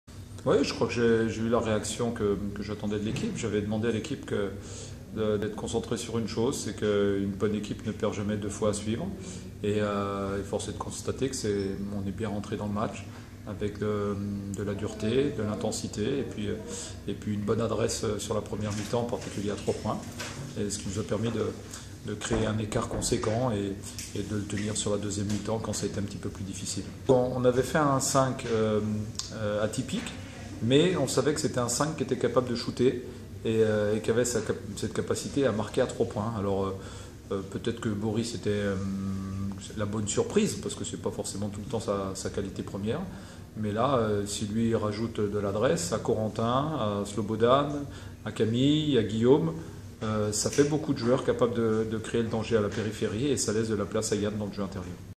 CBC-Vitré (88-78) : la réaction d'Hervé Coudray
Le coach de CBC revient sur la victoire de son équipe qui a su relever la tête contre Vitré (88-78), samedi 1er mars, après la première défaite de sa formation une semaine plus tôt à Tourcoing.